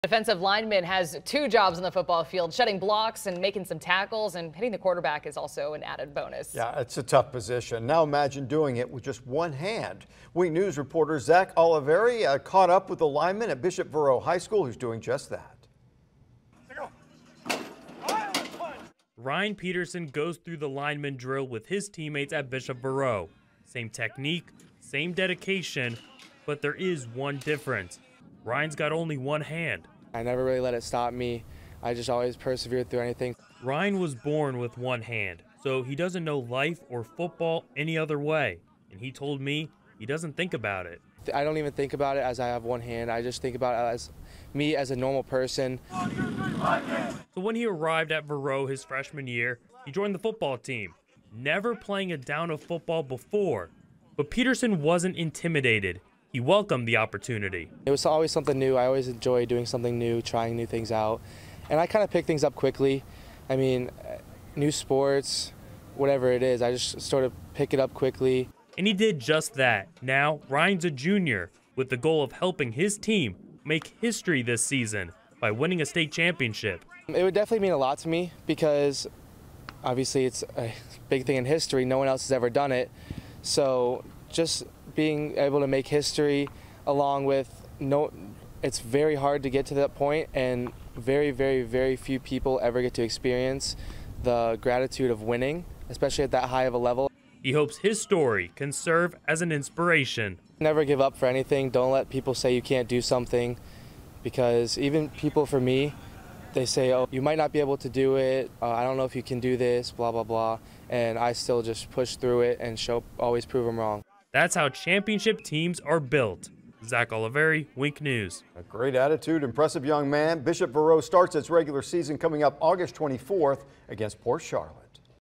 One-handed football player for Bishop Verot hopes to inspire others and win state championship
A defensive lineman's two jobs on the football field are shedding blocks and making tackles, but hitting the quarterback is great extra credit.